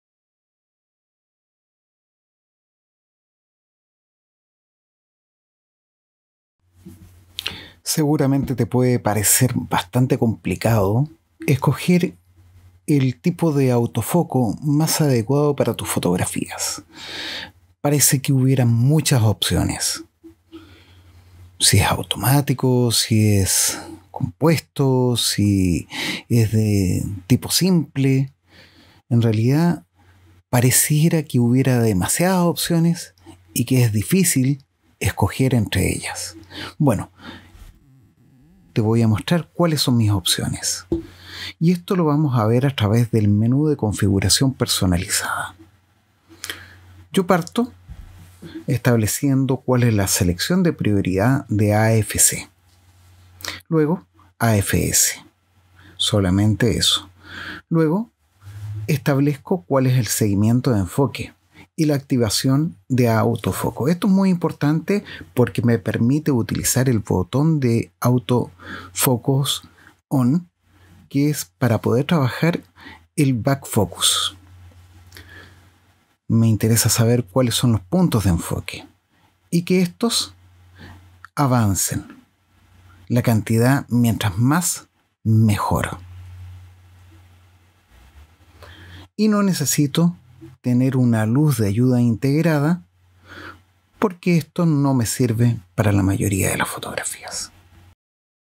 Configura el autofoco en Nikon
En menos de 2 minutos configura el autofoco en Nikon